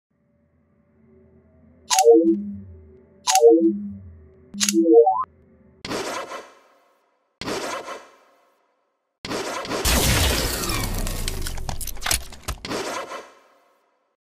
Halo Beam Rifle (Custom Made Rifle Sound Design)
Just a little experiment in Sound Design. I created the sound of the HUD opening and closing, as well as the sound of the rifle firing.

The sound of the HUD opening and closing was all done with Synths combined with the sound of a lighter.

The sound of the rifle is a combination between the same lighter, my voice, me banging on a metal cookie box, a zipper and a considerable amount of digitally distortion, duplicating, pitch shifting, equalization and reverb.

It was a lot of fun to do and a great learning experience for me.

All the physical sounds (that is, not the synths) were recorded with an iPhone mic.